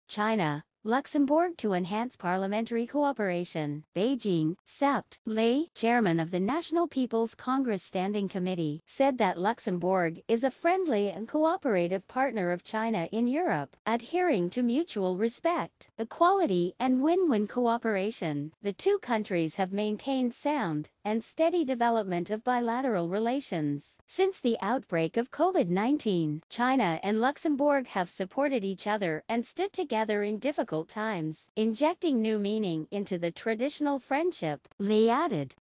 China, Luxembourg to enhance parliamentary cooperation
BEIJING, Sept. 22 (Xinhua) -- China's top legislator Li Zhanshu held talks with Luxembourg's President of the Chamber of Deputies Fernand Etgen via video link Wednesday, with both sides pledging to enhance exchanges between their legislative bodies.$$$Li, chairman of the National People's Congress (NPC) Standing Committee, said that Luxembourg is a friendly and cooperative partner of China in Europe. Adhering to mutual respect, equality and win-win cooperation, the two countries have maintained sound and steady development of bilateral relations.$$$Since the outbreak of COVID-19, China and Luxembourg have supported each other and stood together in difficult times, injecting new meaning into the traditional friendship, Li added.$$$Noting that next year marks the 50th anniversary of the establishment of diplomatic relations between the two countries, Li said China is ready to work with Luxembourg to uphold mutual respect and consolidate mutual political trust. China seeks to promote economic recovery through openness and cooperation, to maintain communication and exchanges for enhancing mutual understanding, and to uphold multilateralism and address global challenges to push bilateral relations to a new level, Li added.$$$Noting that the European Union (EU) is an important force in the world, Li said China will work with the bloc to advance important political agenda through consultation, explore ways to deepen practical cooperation, strengthen coordination in international affairs, and make greater contribution to world peace and prosperity.$$$The Chinese NPC is willing to strengthen exchanges and cooperation with Luxembourg's Chamber of Deputies, maintain high-level exchanges, and promote communications among special committees, Li added.$$$Etgen said that both countries are committed to opening-up, and the development of bilateral ties has seen mutual trust and unshakable solidarity and mutual assistance between the countries.$$$Etgen expressed the hope that bilateral cooperation in economy, trade, finance, equipment manufacturing and other fields will be strengthened. Luxembourg's Chamber of Deputies is willing to deepen exchanges with Chinese NPC and play an active role in promoting bilateral relations. Enditem$$$